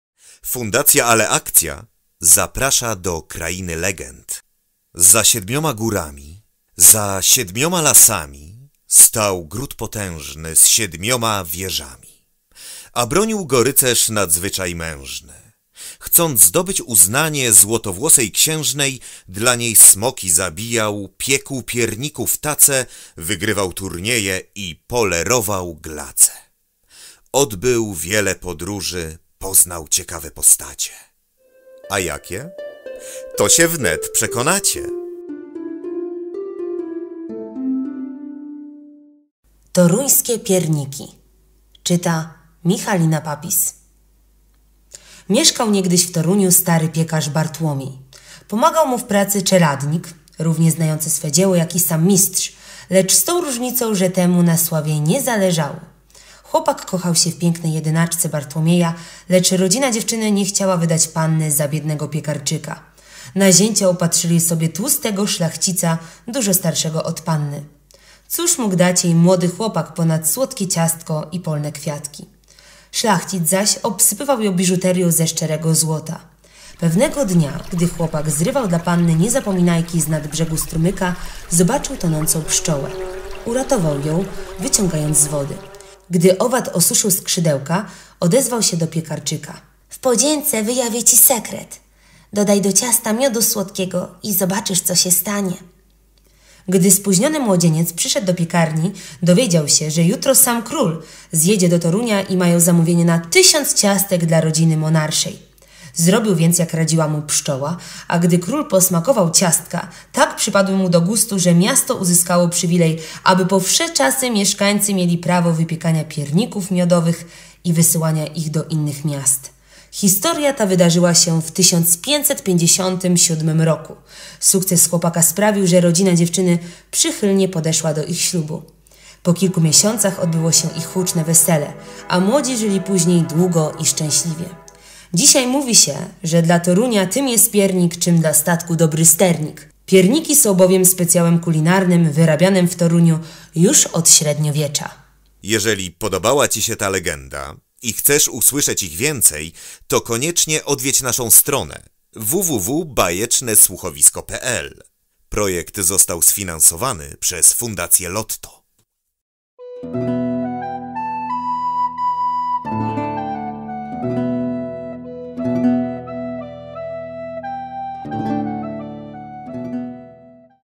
⚔️TORUŃSKIE PIERNIKI⚔️ - KRAINA LEGEND - 🤎BAJECZNE SŁUCHOWISKO🤎
💛KRAINA LEGEND💛 - AUDIOBOOKI Z POLSKIMI LEGENDAMI DOSTOSOWANE DO POTRZEB DZIECI NIEWIDOMYCH I NIESŁYSZĄCYCH

🛡TORUŃSKIE PIERNIKI🛡

Czyta: Michalina Papis
Grafika/Animacja: Norbert Zbiciak
Montaż/dźwięk: Michalina Papis
Opracowanie: Ewa Szyszkowska - Papis
Lektor: Erwin Piwowarczyk
Wydawca: Fundacja Ale Akcja!

⭐PROJEKT ZOSTAŁ SFINANSOWANY PRZEZ FUNDACJĘ LOTTO⭐